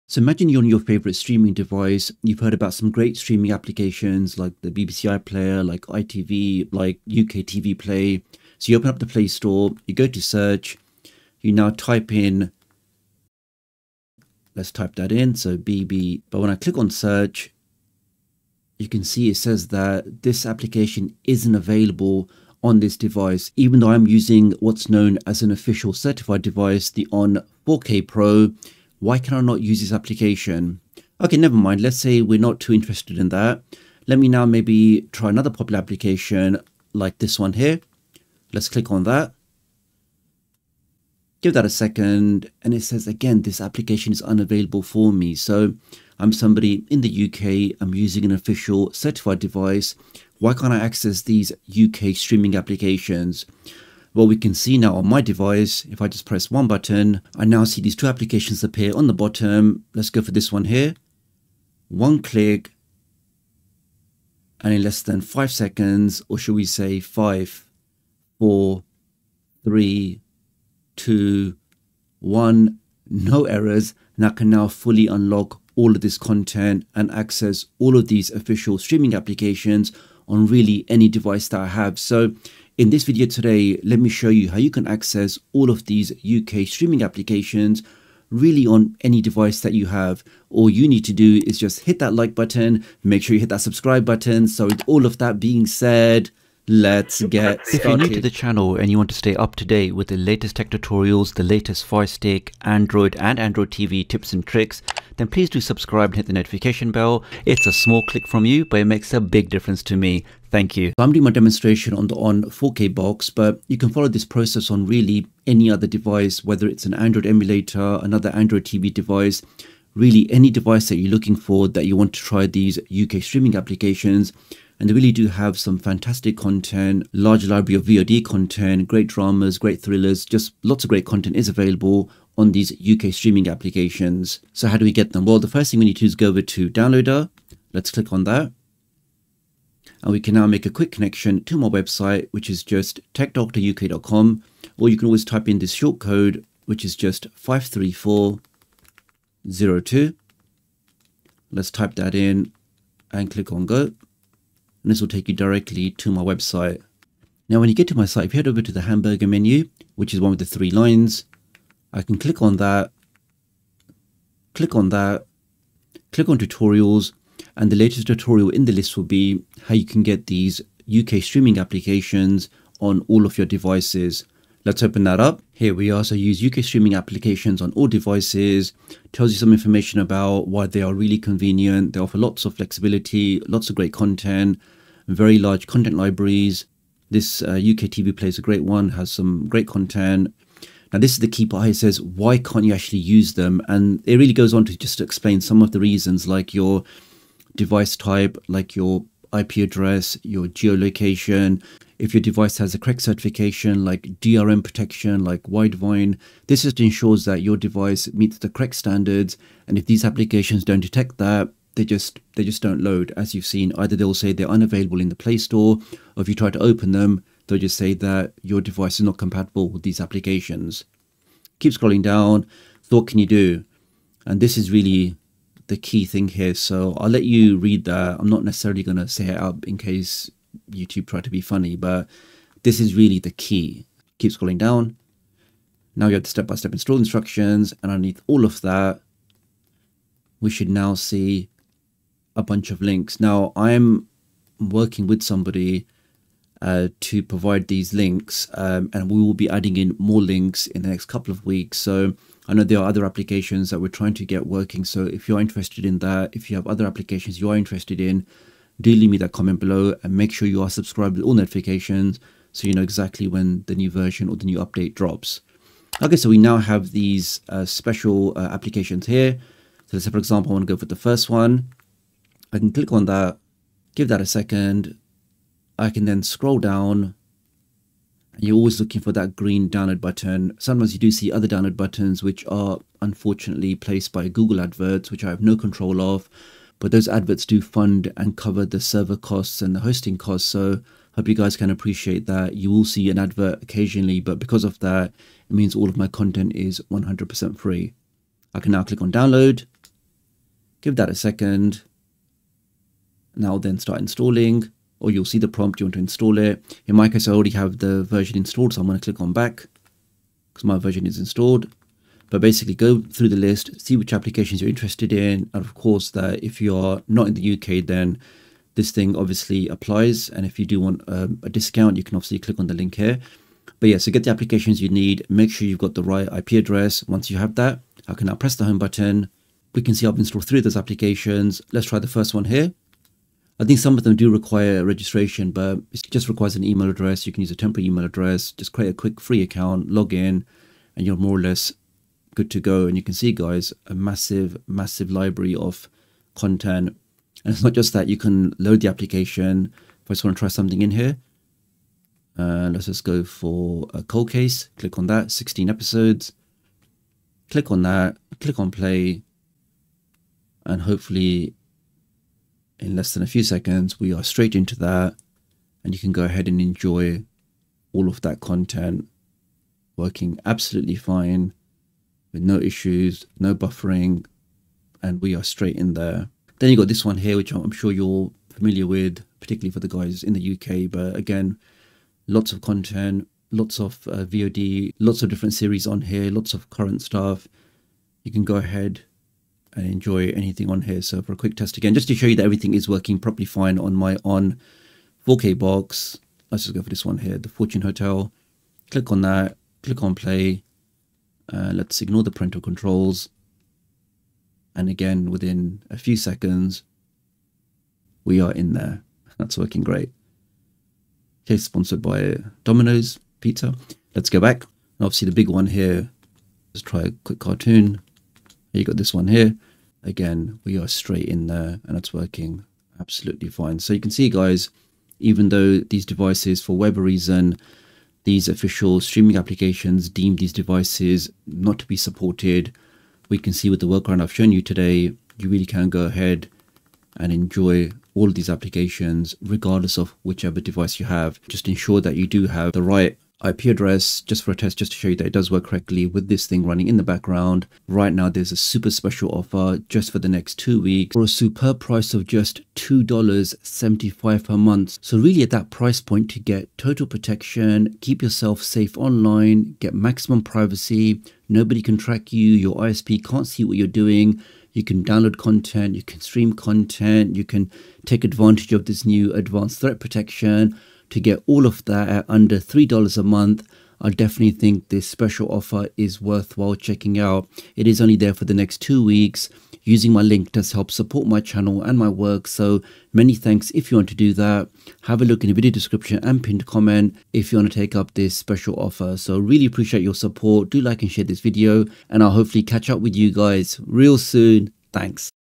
🔴INSTALL THE BEST STREAMING APPS ON ALL DEVICES !
Trying to use Streaming apps on Android TV or another unsupported device? Watch this video on how to use UK Streaming APPs on Any device !
Best Streaming VPN For 2024:
for the pic of the internals